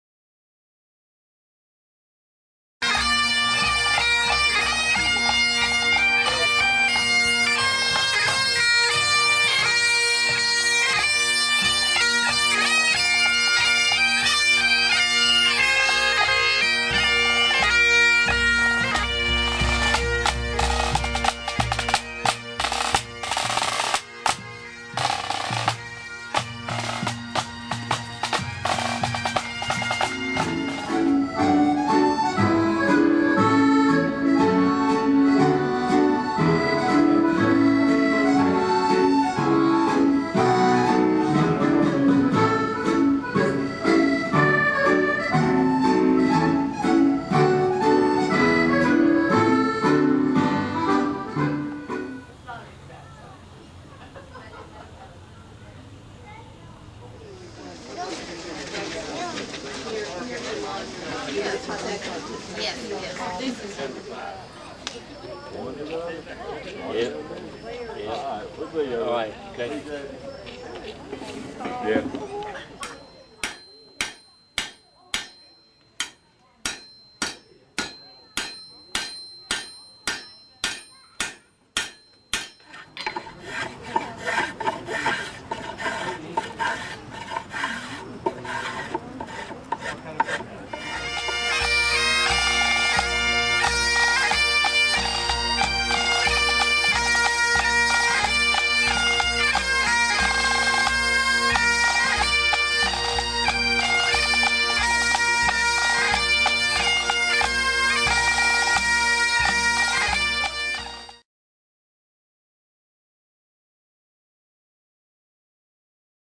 Schiele Scottish Heritage Day
Video from the Schiele Museum's annual Scottish Heritage Festival held Sunday.